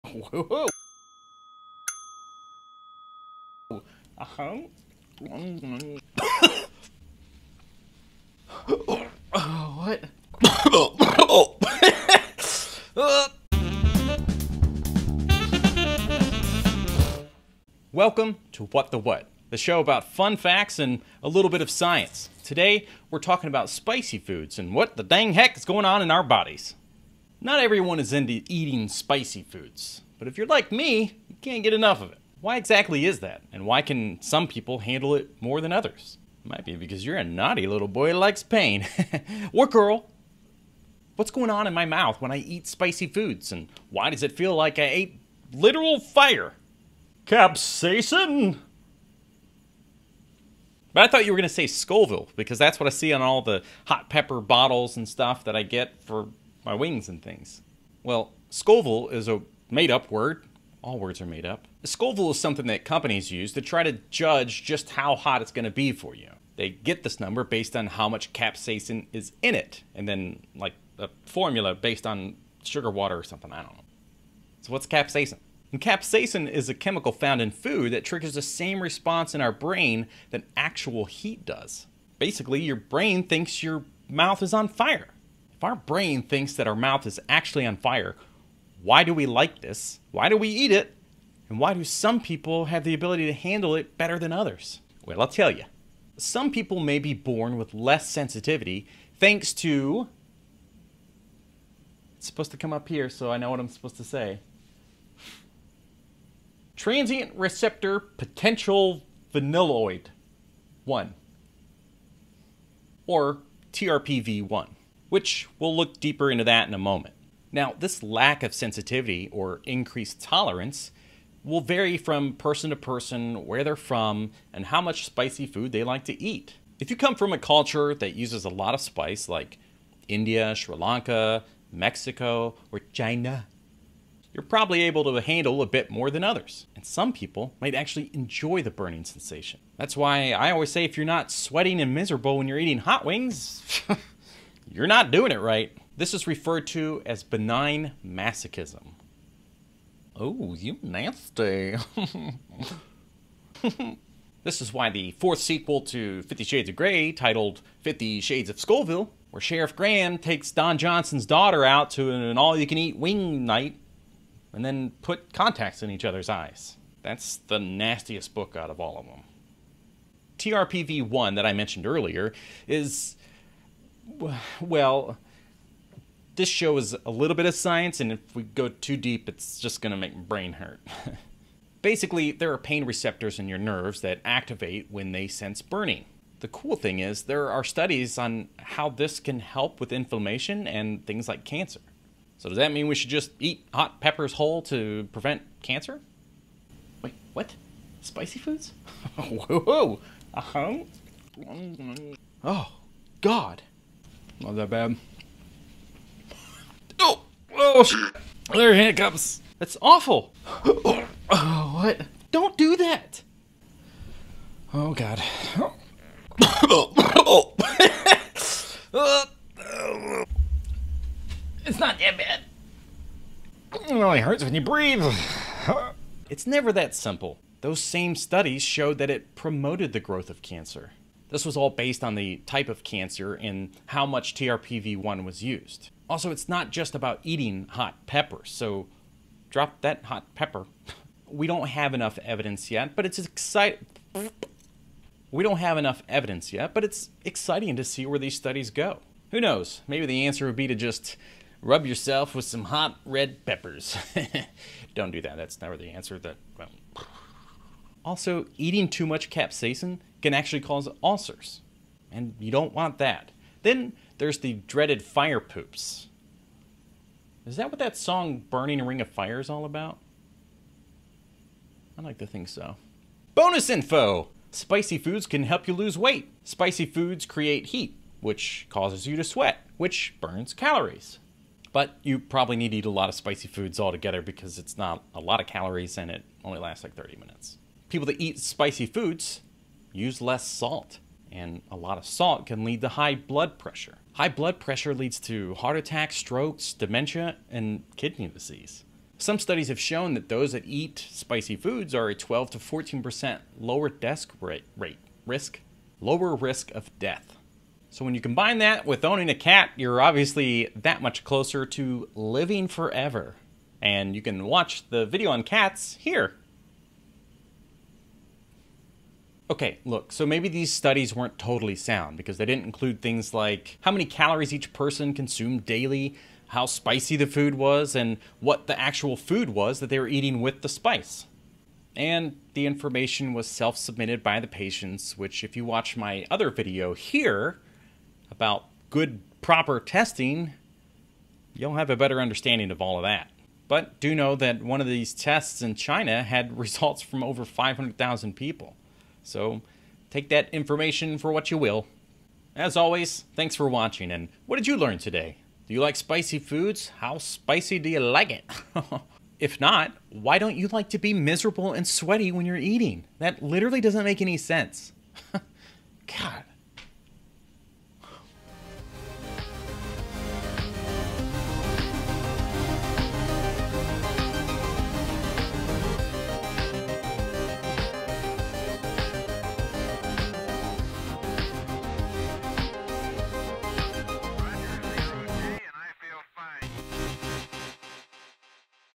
Spicy Foods - What is Capsaicin?
What’s happening in your mouth when you eat spicy foods? Why does it feel like my mouth is literally on fire? Capsaicin! And Capsaicin is a chemical found in spicy foods that triggers the same response in our brain that actual heat does. That means your brain thinks your mouth is on fire!

Some people may be born with less sensitivity thanks to Transient Receptor Potential Vanilloid 1, or TRPV-1. Basically, they are pain receptors on your nerves that activate when they sense burning.

[Episode was written by Jonathan Maxwell]
This is a weekly show with new episodes coming every Sunday.